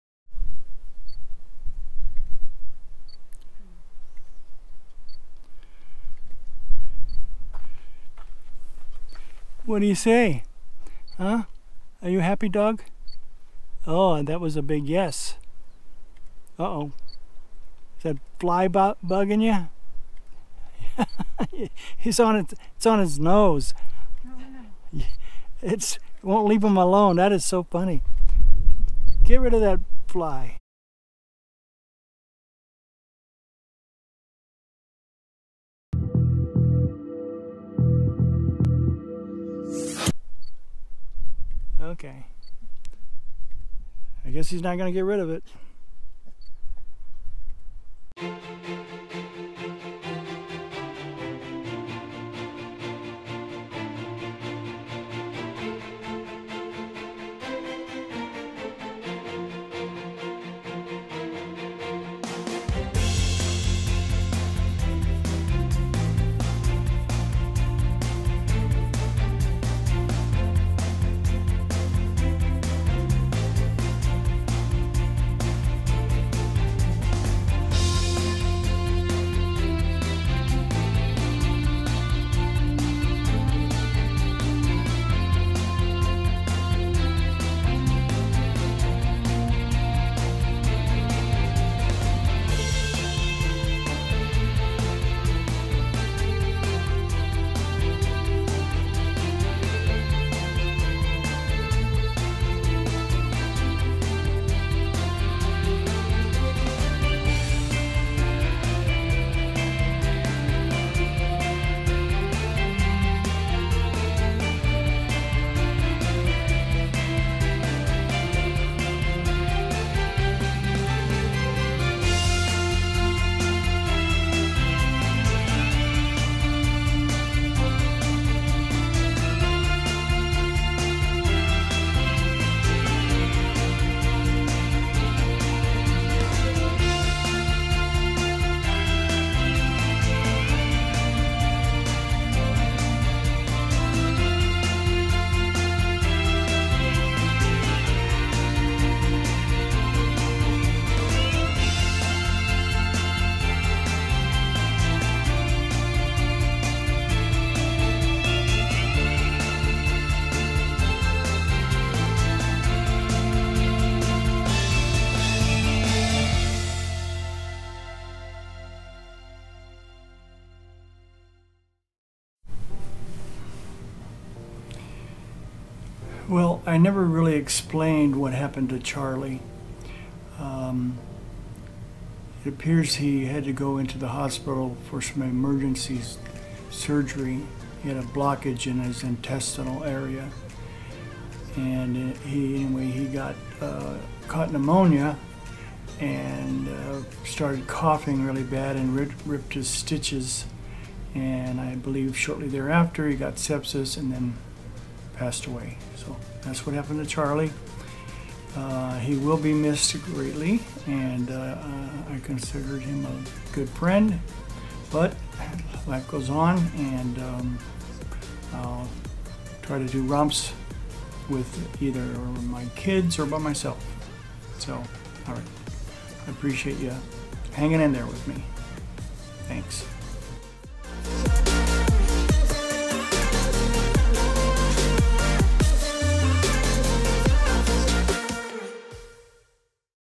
Drones in the forest and some explaining. 127.
A little "romp" with Bailey and my son, Morgan. I also explain what happened to Charlie.

All flights comply with the FAA rules and regulations.